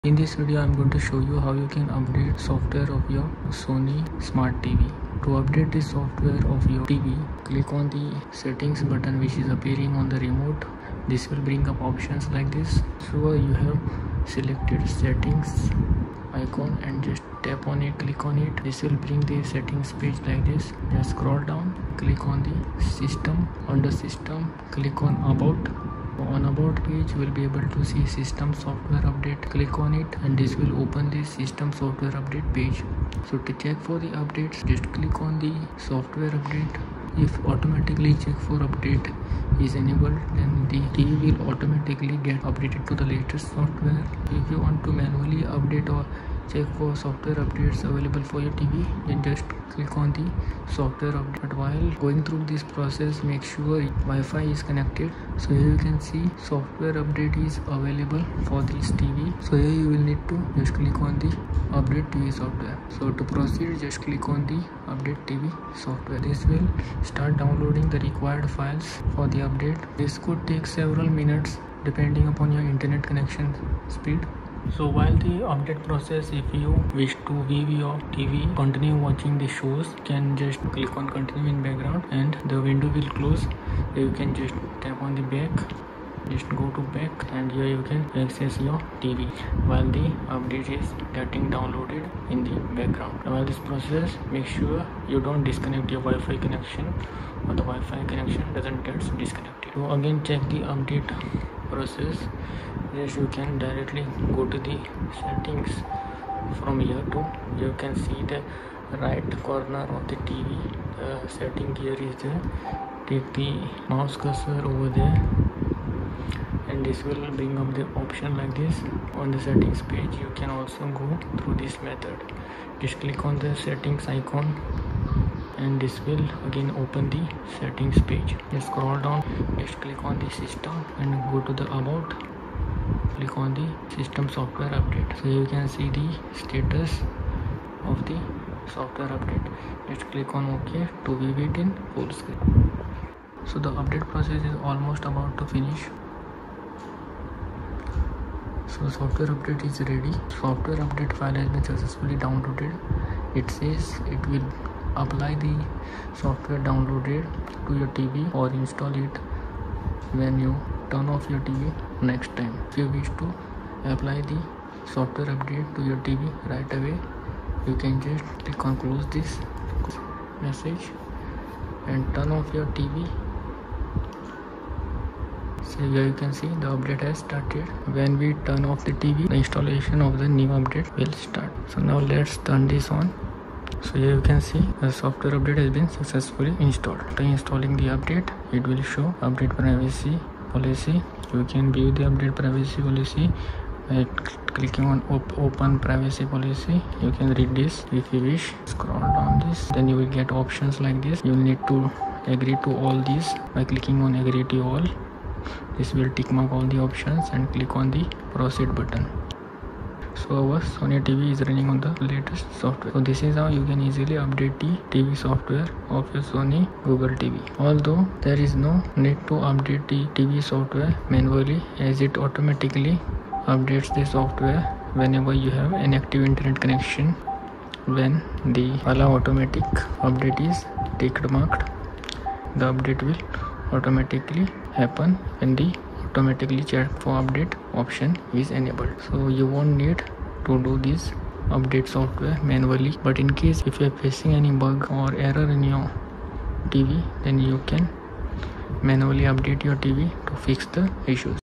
Sony Bravia Google TV Software Update
This video shows the process to be followed to perform Online Software Update on Sony Bravia Google TV.